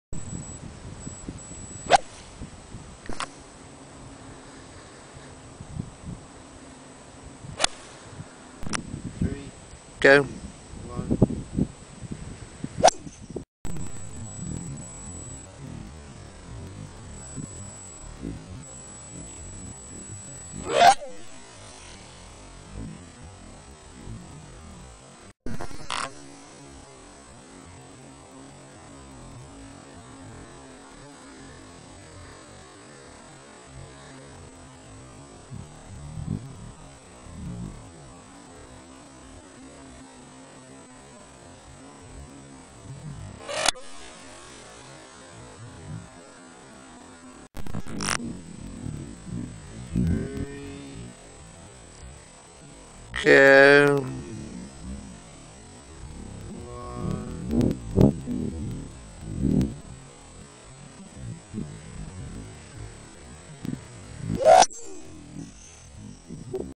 Golf Swing
Found some random videos from a golf day 3 years ago... this is my swing.